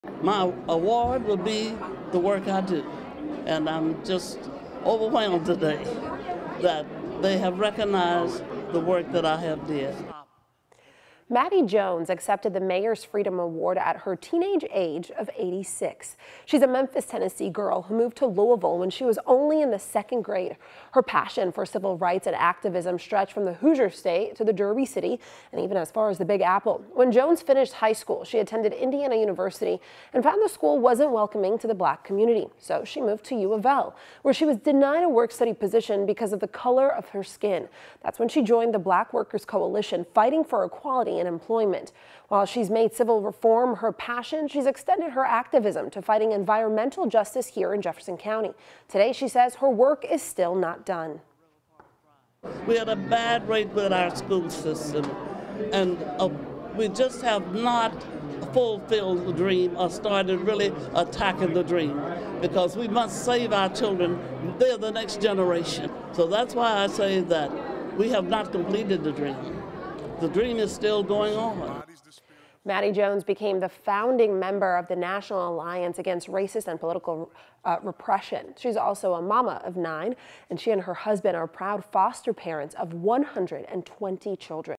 Civil rights pioneer Mattie Jones honored with 2020 Dr. Martin Luther King Jr. Freedom Award
Jones, who describes herself as “just another soldier in the army for peace, justice, and equality,” said she is honored by the award.